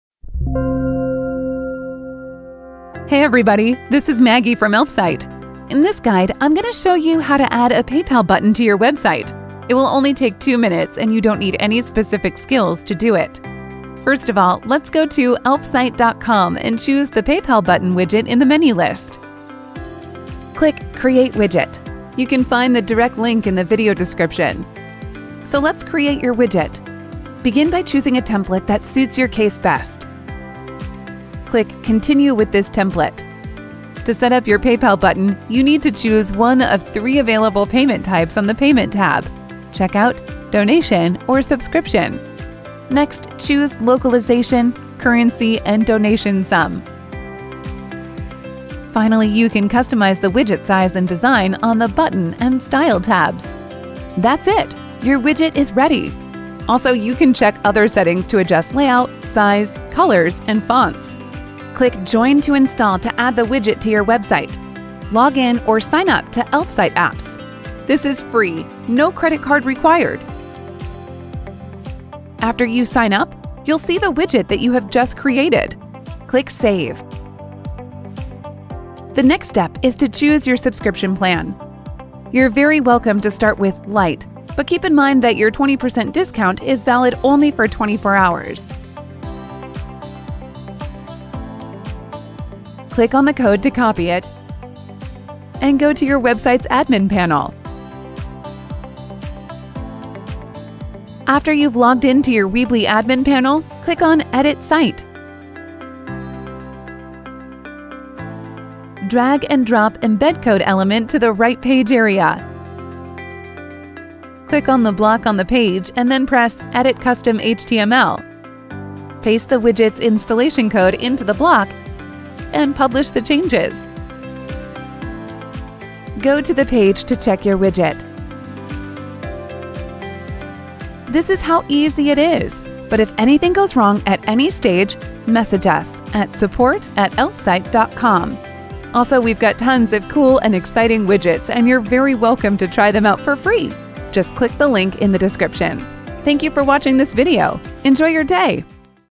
How to Add PayPal Button App to Weebly
PayPal Button is a free tool to include one of the most reliable payment systems to your website. Facilitate paying process, save effort and time.

Benefits:

• Trusted worldwide payment method on your website.
• Styling and adding in no time.

To embed our app, see all the steps from the detailed guideline below:

1. Build and configure the app;
2. Save a unique code from the popup;
3. Paste the saved code on the website.

Click on the link below in order to find out how to embed Weebly PayPal Payment plugin -

Example 1: Checkout button can be set to include tax and shipping fee.
Example 3: Subscription button features a recurring option and flexible period|regularly renews payment after the period ends.

Here’s how simple it is to add PayPal Payment plugin to Weebly! If you want to learn more about elements, adjustments, and app templates, you can write to our experienced Support team. We’ll be happy to help you!